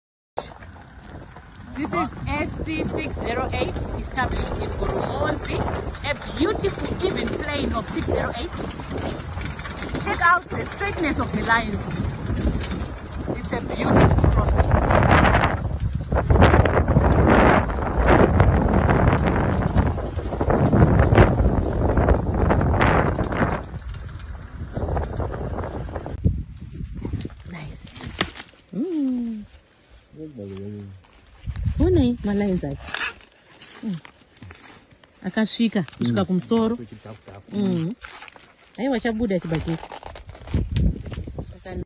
SC 608 yellow maize established off Shamva road in Zimbabwe
SC 608, yellow maize hybrid with an even crop plan and extra large sized cobs🌽🌽 🌽